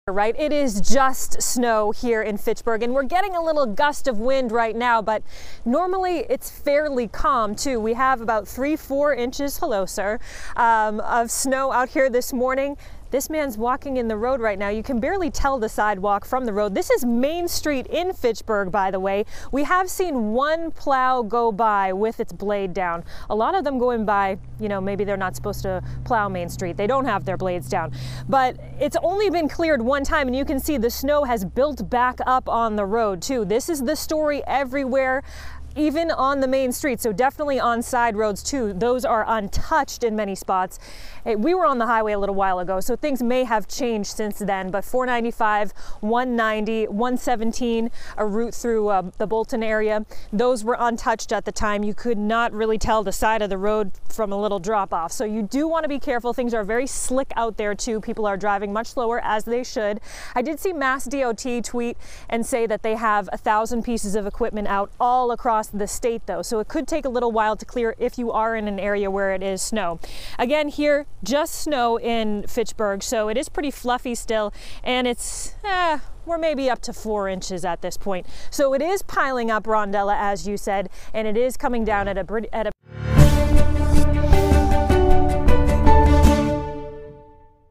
Heavy snow before rain switchover north-west of Boston
It's been mostly snow for areas north and west of Boston.